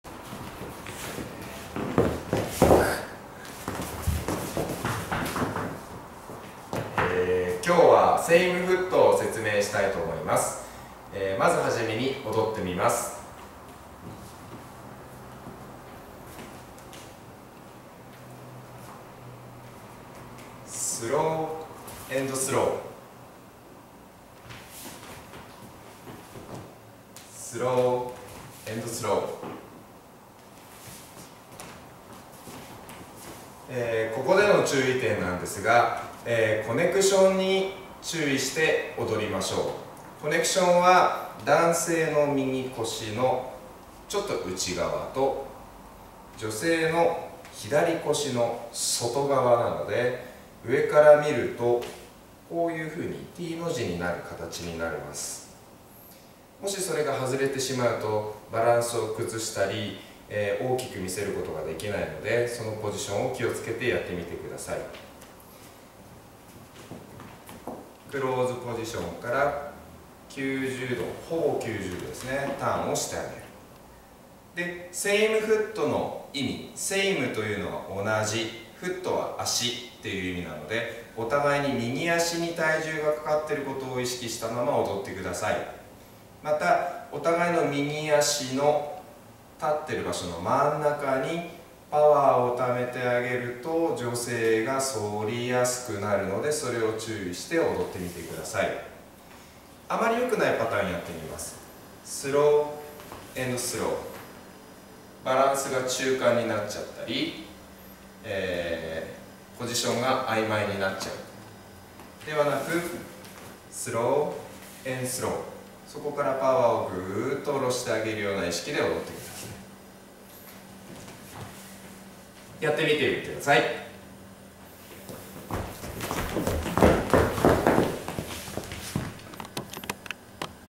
タンゴ セイムフット 山嵜＆石本組
タンゴ　セイムフット　山嵜圭太＆石本美奈子組
エムズ・ダンスアカデミーワンポイントレッスン動画

メルマガにて毎週火曜日にダンス動画無料配信中！

★制作★
日本最大級・最高レベルのダンス教室
詳しくは【エムズダンス】で検索！！